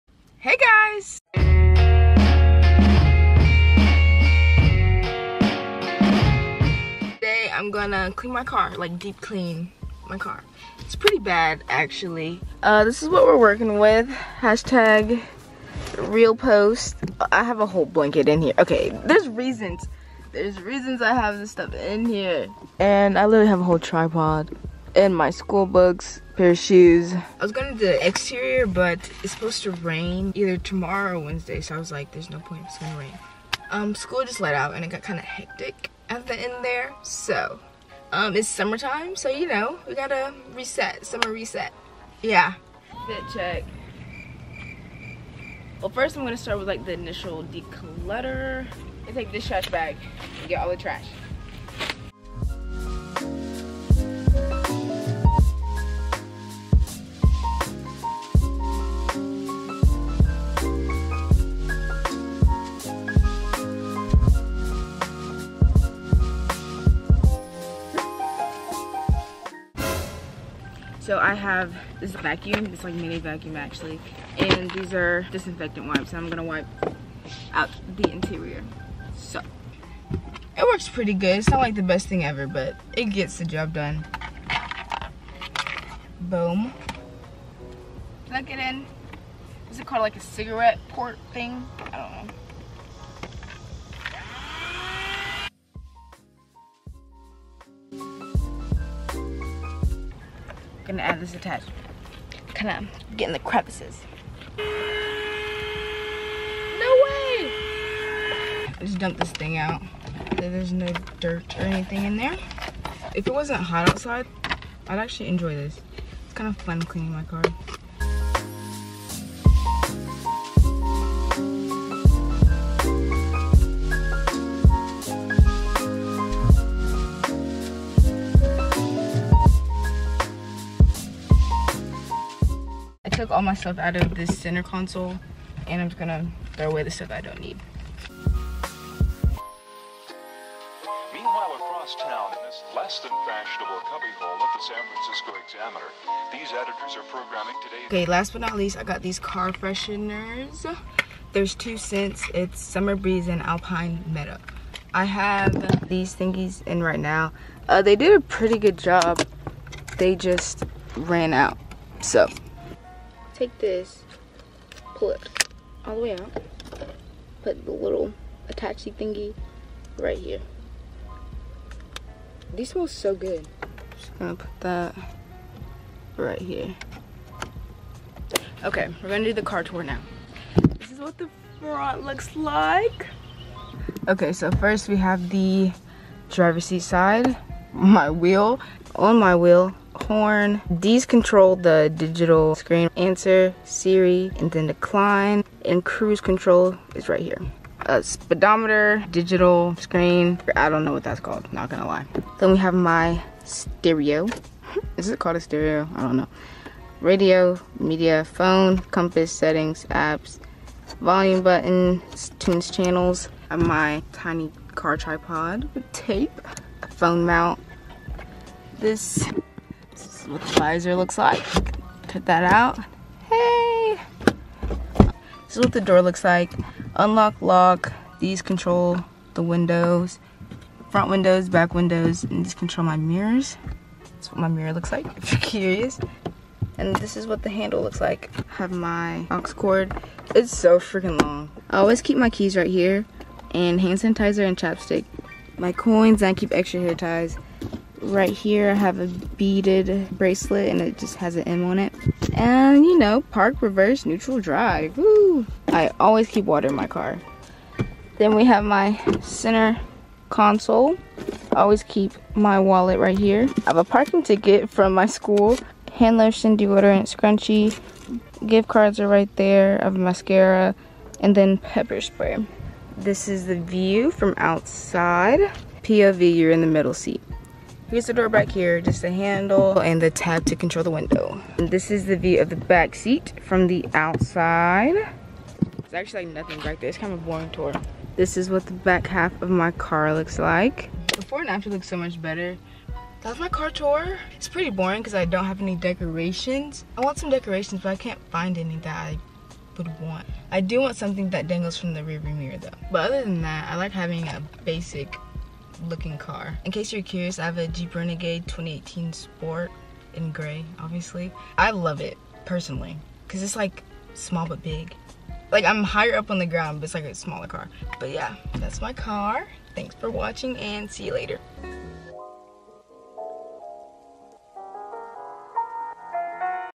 CLEAN MY CAR WITH ME + CAR TOUR !
hey! today i cleaned/decluttered my car and gave you guys a car tour.

car model: jeep renegade sport 2018